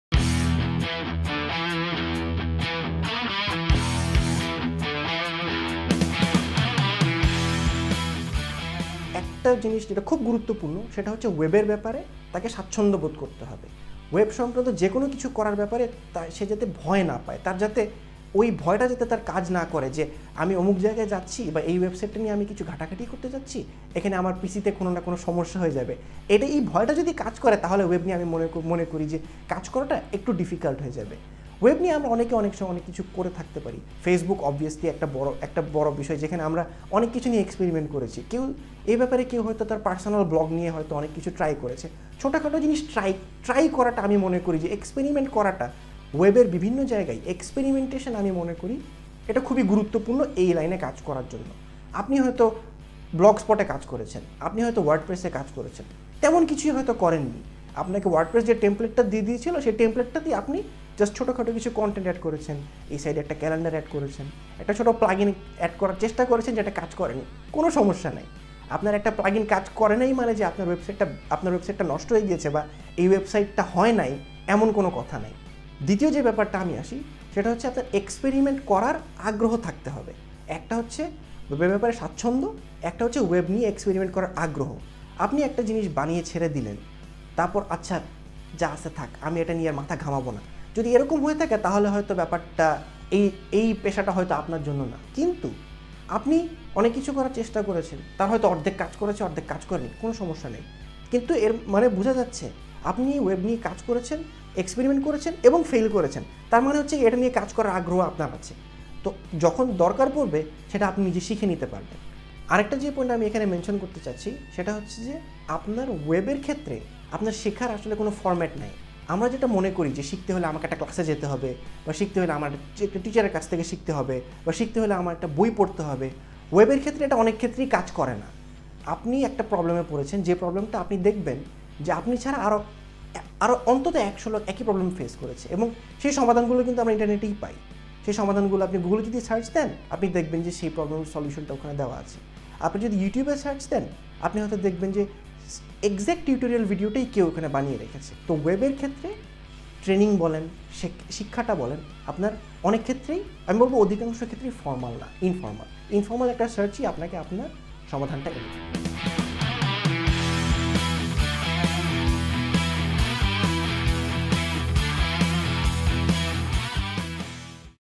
How to be a Web Developer: ওয়েব ডেভেলপার হওয়ার জন্য তিনটি পরামর্শ
ওয়েব ডেভেলপমেন্টে নিজের ক্যারিয়ার গড়ে তোলার জন্য খুব সহজ তিনটি পরামর্শ দিয়েছেন বিবিসি মিডিয়া অ্যাকশনের নাশিদ আশরাফ।

JobSixer presents career development video
"How To Be A Web Developer"
Important tips by BBC Web Producer Nashid Ashraf